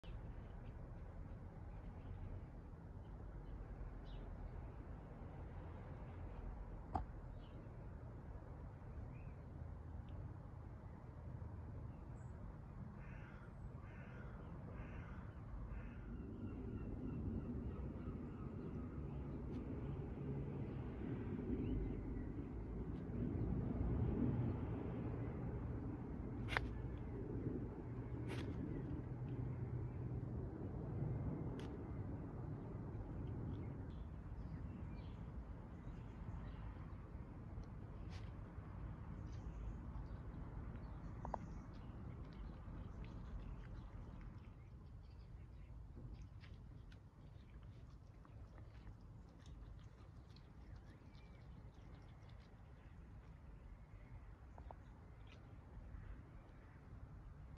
WILD FOX SNAPS AT FOOD BUT LEARNS TO BE GENTLE
Philip fox isn't used to human interactions but foxes are very trusting very quickly